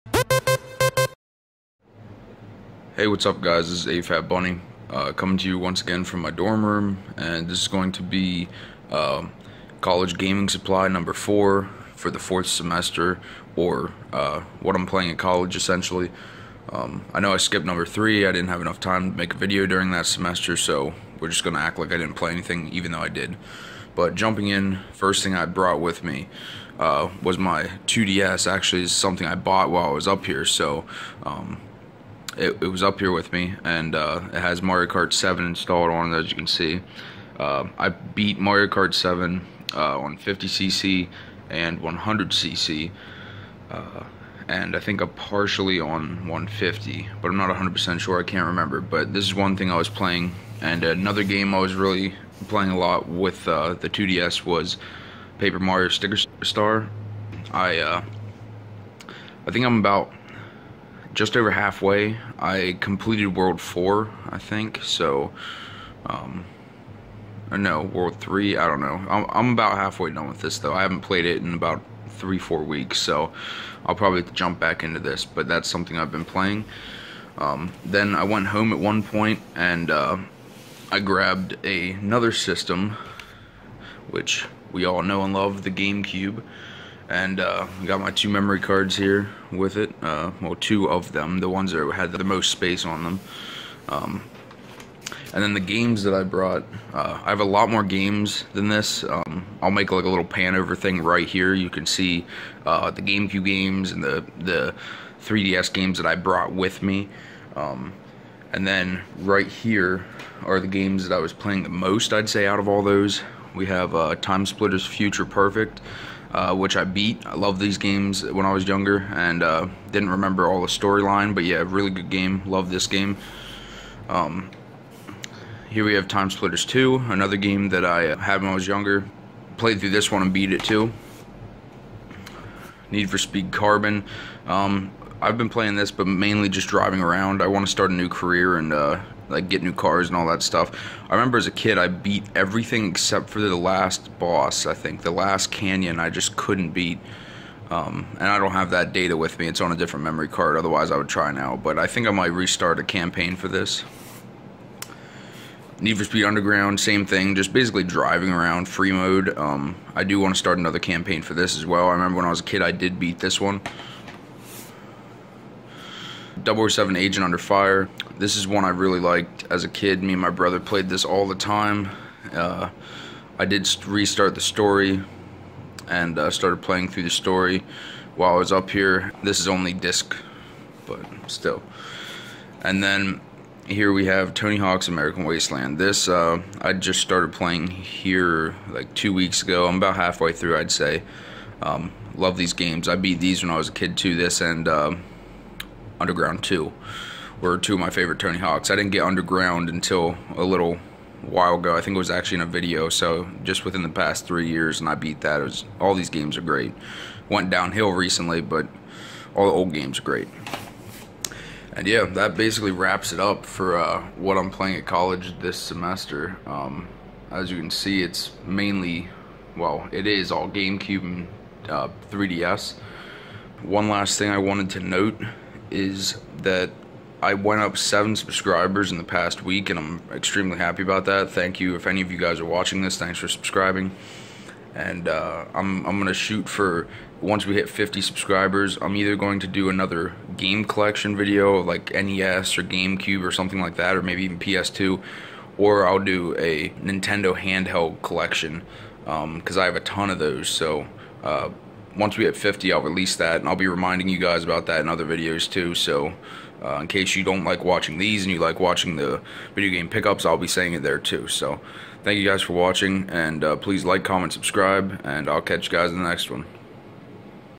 College Gaming Supplies - 4th Semester
This video shows what games I am currently playing at college.